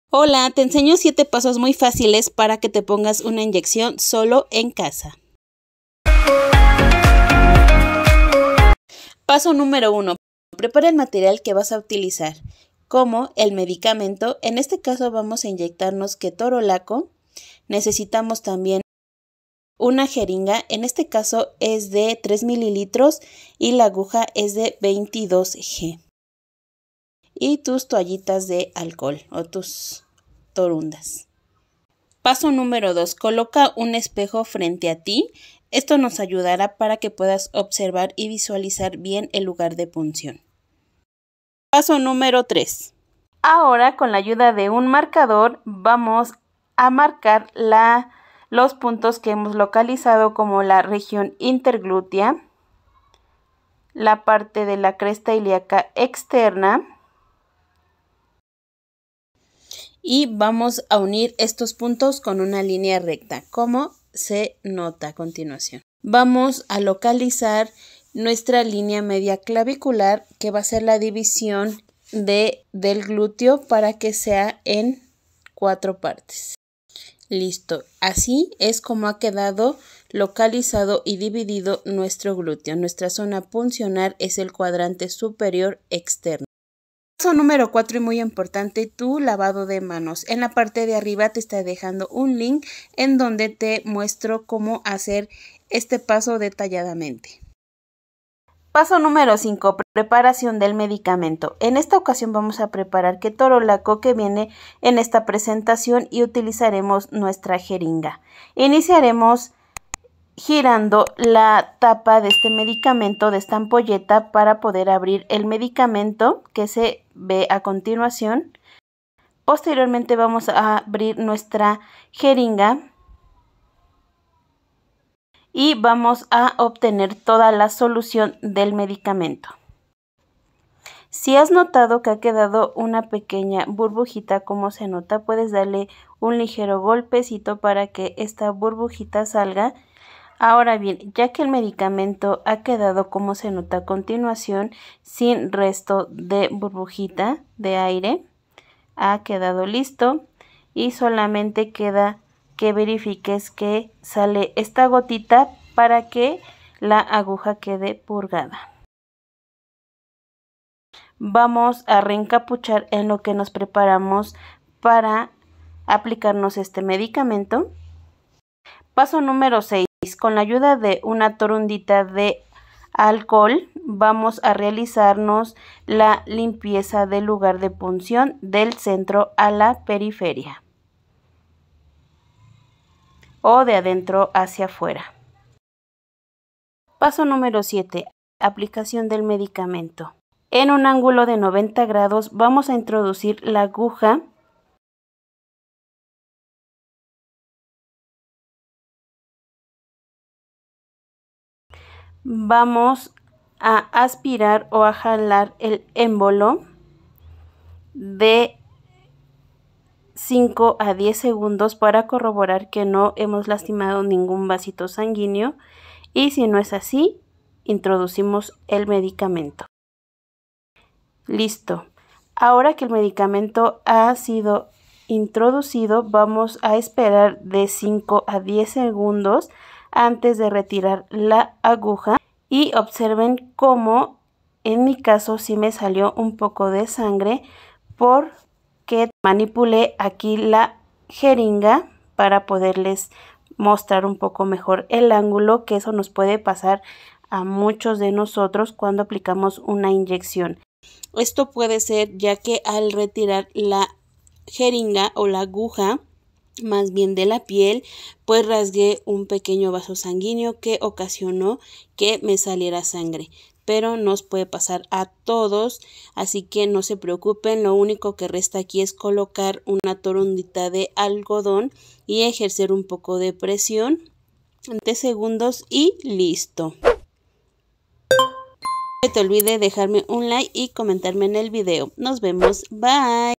✅7 PASOS para una INYECCIÓN "INTRAMUSCULAR" SOLO en CASA 🛑SIN DOLOR🛑✅
🌟7 pasos para una inyección intramuscular en el glúteo sin DOLOR
Te muestro como realizarlo super fácil en casa así que no te lo pierdas espero te ayude un montón saludos🌟
paso 1: 0:09 prepara el material a utilizar
paso 2: 0:33 coloca un espejo para ayudarte a localizar el área a puncionar
paso 3: 0:42 división del glúteo
paso 4: 1:28 lavado de manos
paso 5: 1:39 preparación del medicamento
paso 6: técnica 2:49
paso 7: aplicación 3:08
MIRA TAMBIEN ESTOS VIDEOS:

ESPERO QUE PUEDAS VER ESTOS VIDEOS SALUDOS A TODOS:)